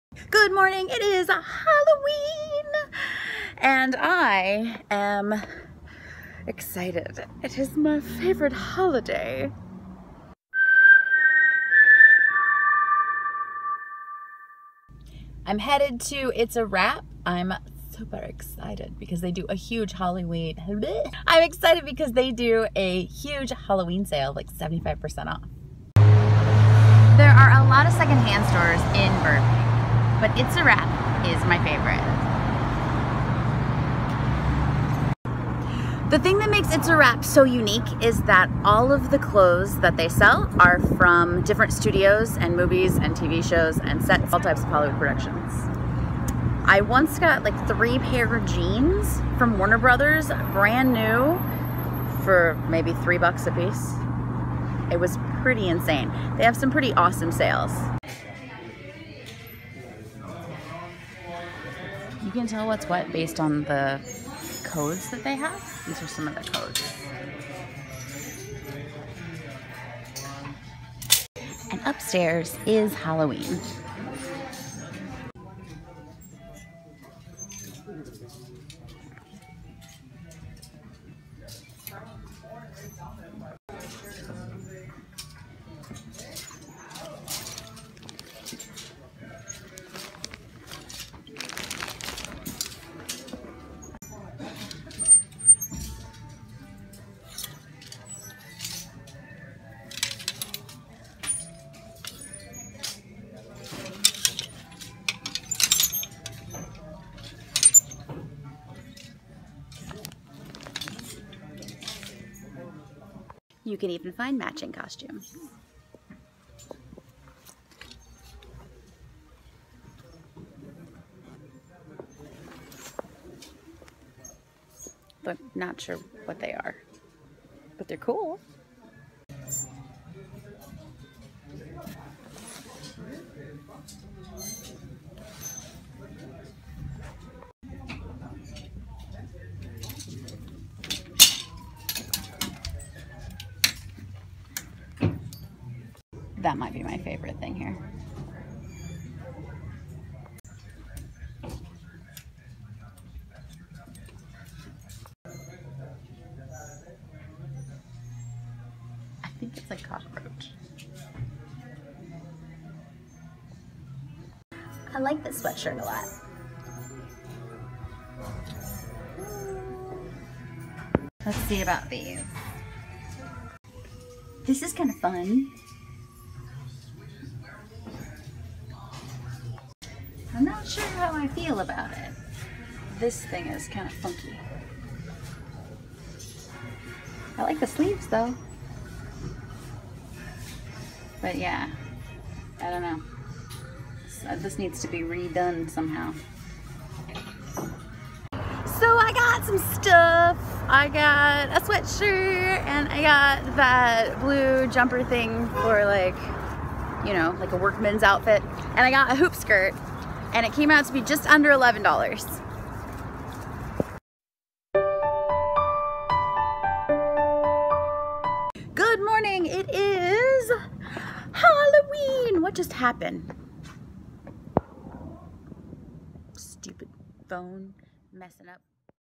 Halloween costume shopping at It’s a Wrap used movie wardrobe store
It’s A Wrap in Burbank California sells used clothing from movies and TV shows.

Instagram ChristineBargerOfficial
Twitter Musical.ly Snapchat ChristineBarger
Live.me PuppetGirl